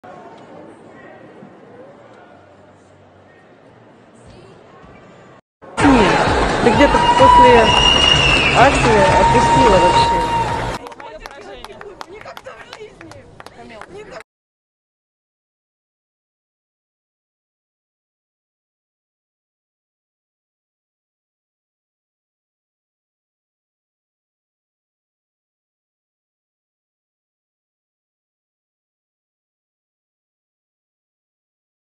KAMILA VALIEVA breaks down in TEARS | Criticized by Coach Eteri | Hugged by VOLUNTEER after falling
Kamila Valieva falls twice in free skating routine at Beijing Winter Olympics